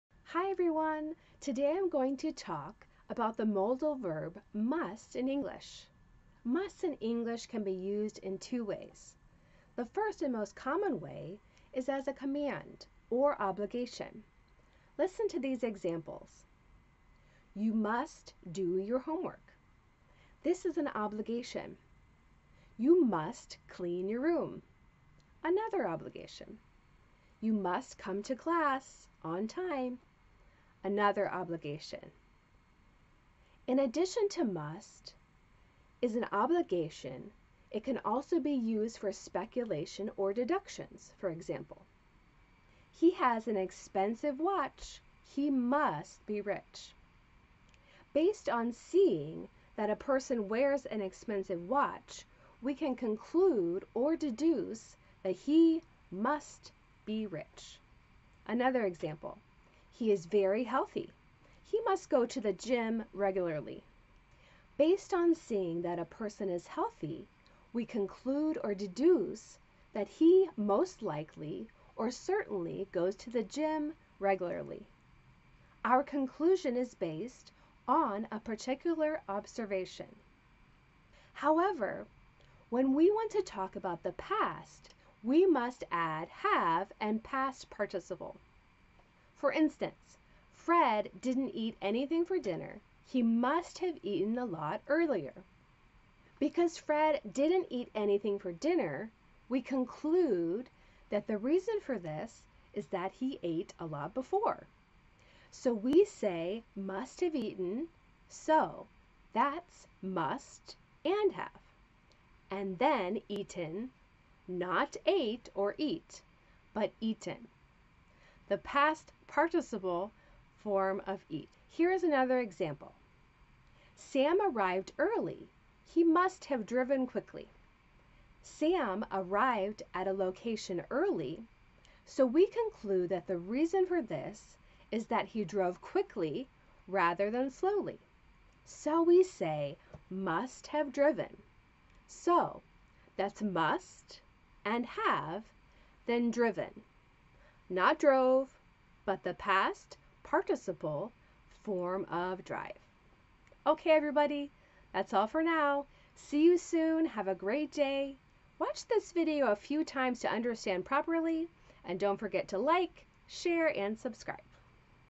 Use of Must in English | The different uses of the word 'must'
Listen to Rebecca explaining the different uses of Must in English and how to use the word 'Must'

An experienced native-speaking ESL teacher from America!

'Must' can be used for obligations.

It can also be used for deductions.